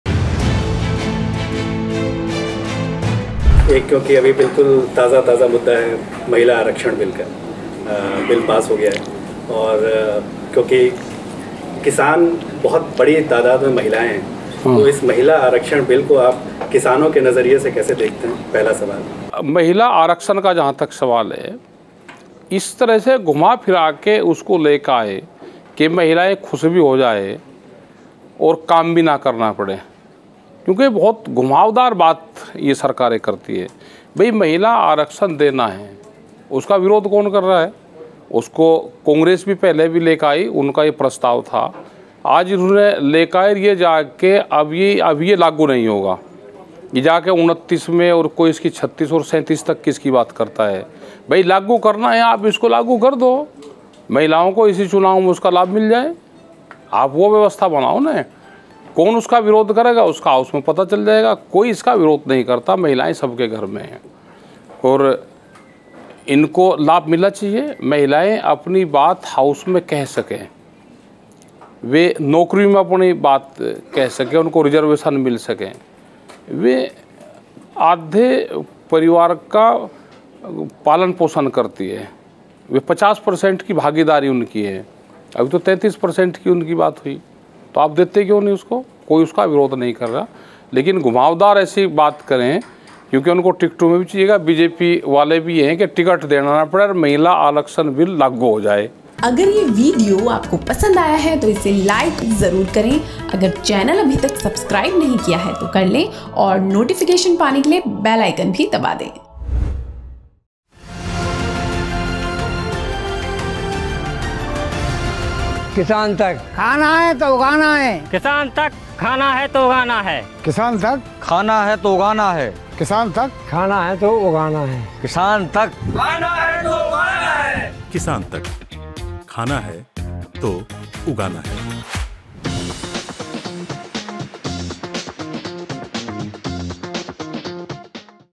Rakesh Tikait:'महिला आरक्षण बिल पर मोदी सरकार की मंशा कुछ और' महिलाएं 33 नहीं 50% की हकदार |KisanTak
महिला आरक्षण बिल पर राकेश टिकैत ने कहा कि हम उसका समर्थन करते हैं और मानते हैं कि महिलाएं 33 नहीं बल्कि पूरे 50% की हकदार हैं. राकेश टिकैत ने मोदी सरकार पर निशाना साधते हुए कहा कि वो सच में महिलाओं के सपोर्ट में नहीं बल्कि वोट के लिए घुमावदार बातें करते हैं.